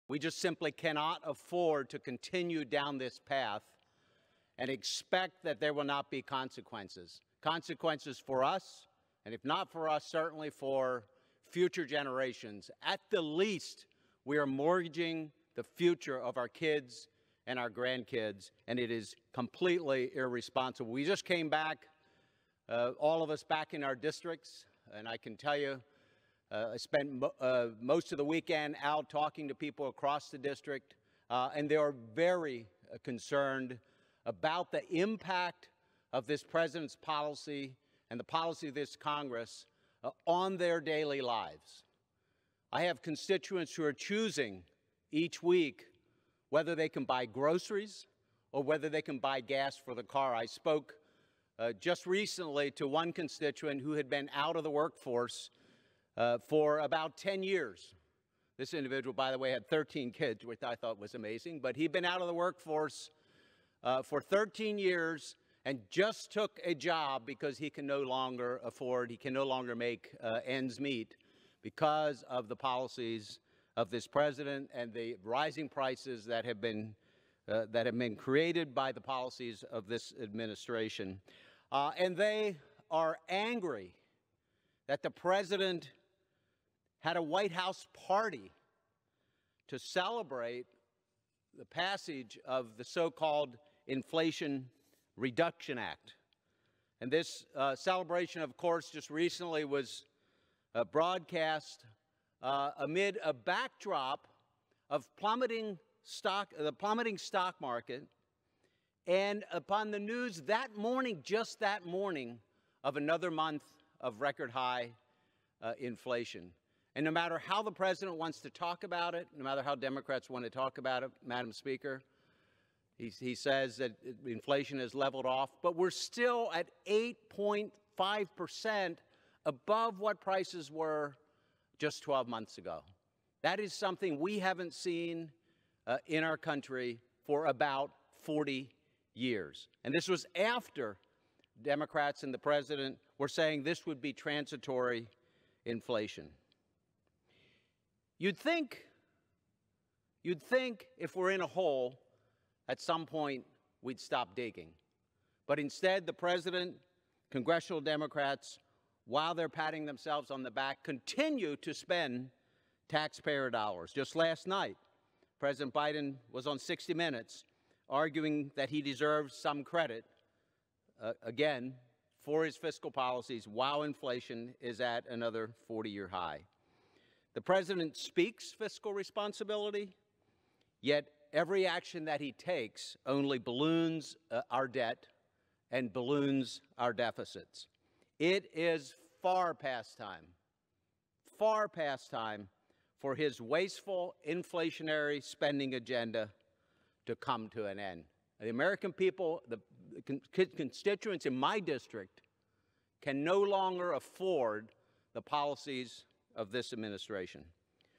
Rep. Smucker: "President Biden is Out of Touch with the Failing Economy"
Rep. Smucker offers comments from the floor discussing the Democrats' so-called "Inflation Reduction Act."